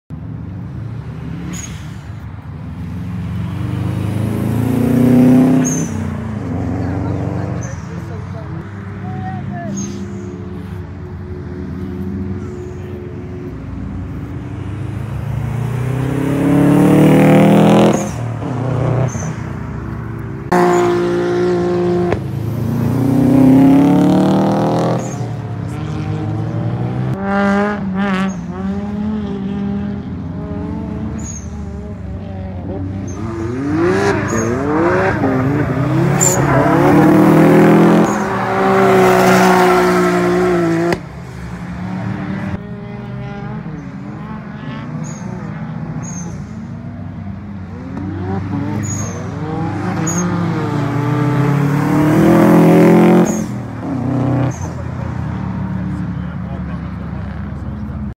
Subaru Impreza WRX - Kakucsring
Örömautózás beteg gyermekeknek 2019 December